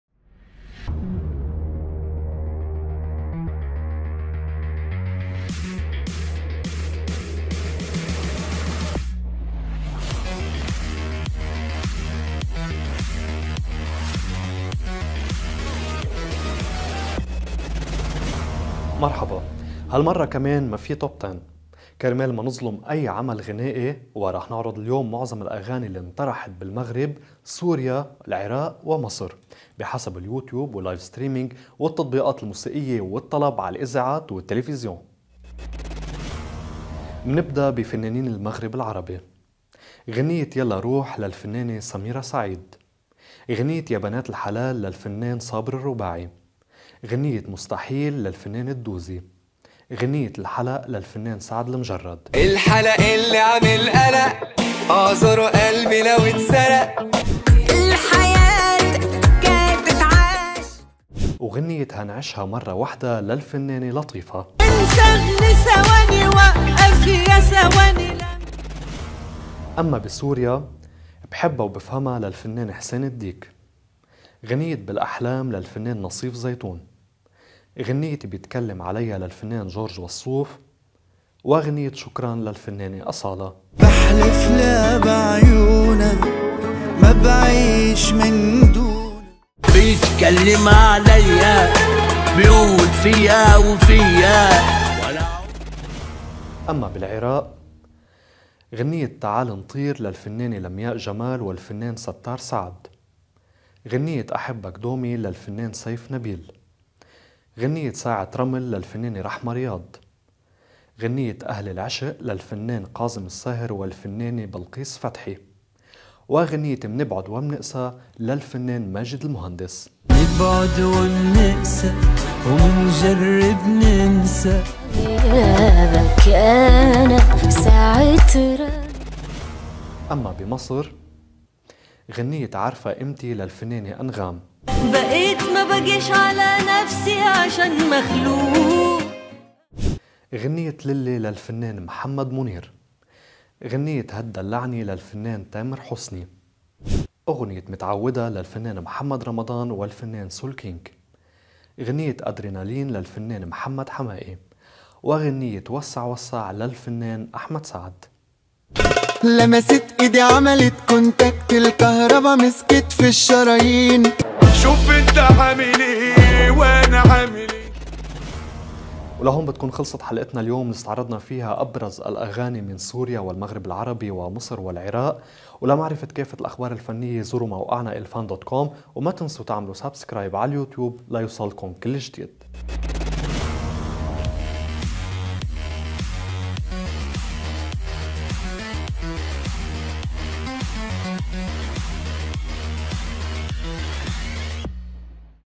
أصالة تُفاجئ الجمهور في عام 2022..وهذا ما فعله سعد لمجرد ومحمد رمضان وغيرهما
يطل موقع "الفن" عليكم بحلقة جديدة وخاصة تتضمن أبرز الأغاني التي طرحت في مصر وسوريا والمغرب العربي في عام 2022، والتي نالت أعلى نسبة مشاهدة على يوتيوب واللايف ستريمينغ، ونسب الإستماع على التطبيقات الموسيقية والطلب على الإذاعات والتلفزيون، وذلك بعد أن كنا في السابق نطلّ عليكم كل أسبوع من برنامج "توب 10 الفن" الذي نعرض فيه سباق أهم الأغاني في مرحلة معينة على الساحة الفنية.
وقد تضمنت هذه الحلقة الكثير من الأغاني لأهم الفنانين، تتعرفوا عليها في هذا الفيديو المرفق بالخبر.

Copyright : ElFann.com